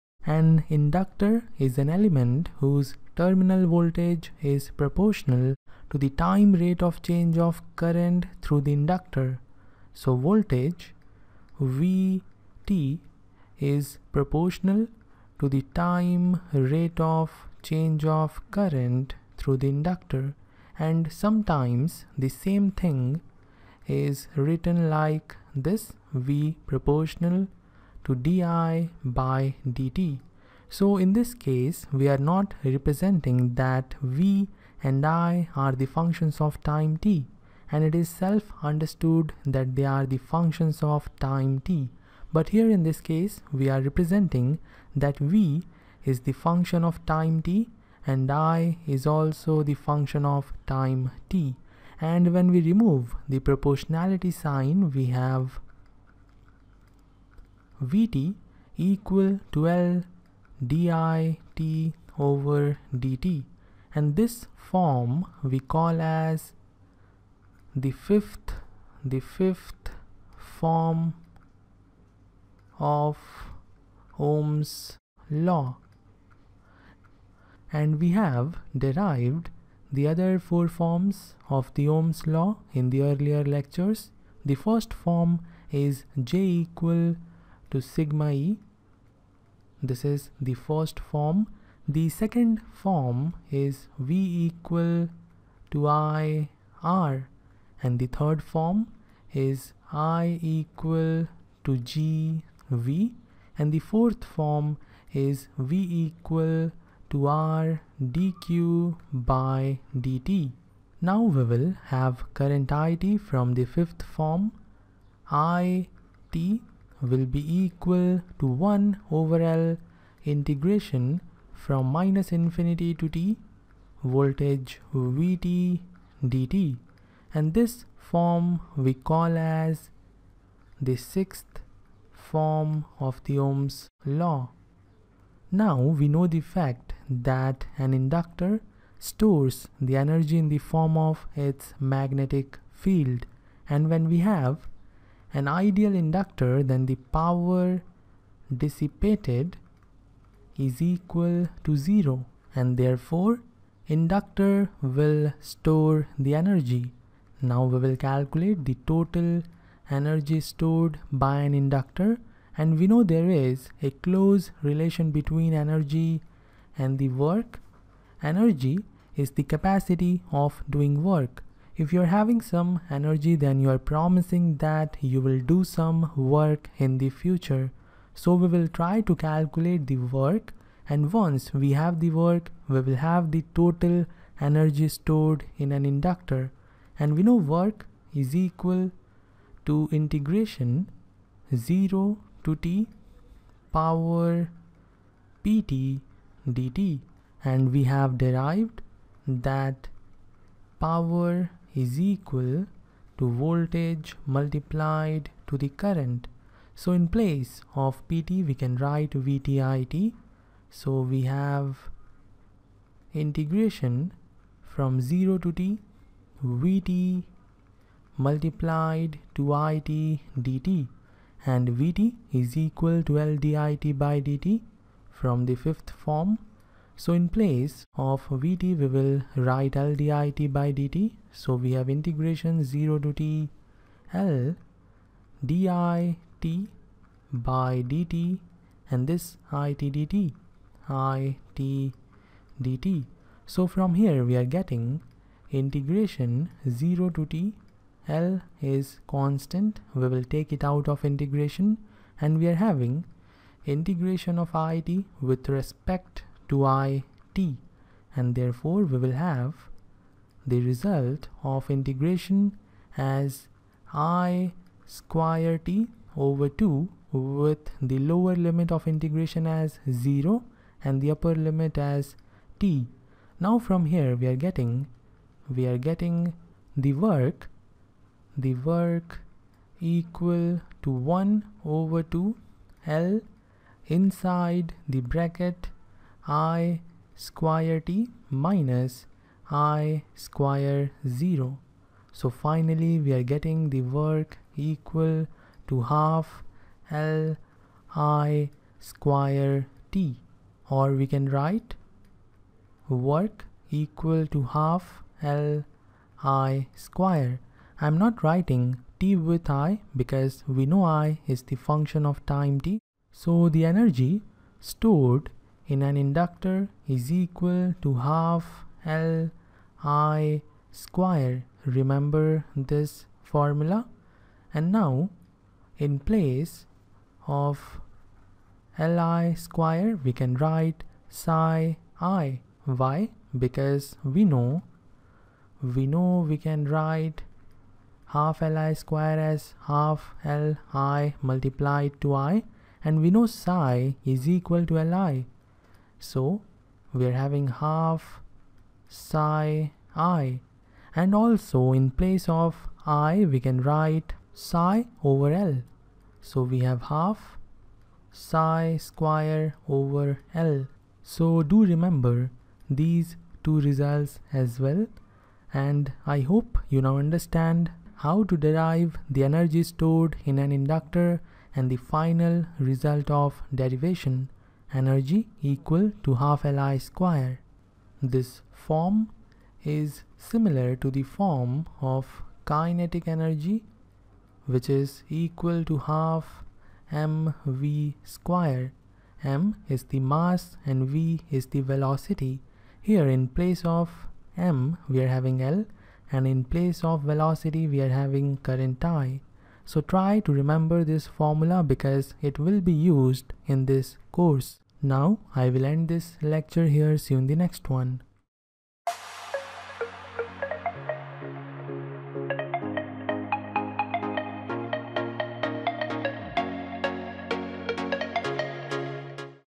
Energy Stored in an Inductor
Network Theory: Energy Stored in an Inductor
Topics discussed:
1) The fifth form of Ohm's law.
2) The sixth form Ohm's law.
3) Energy stored in an inductor.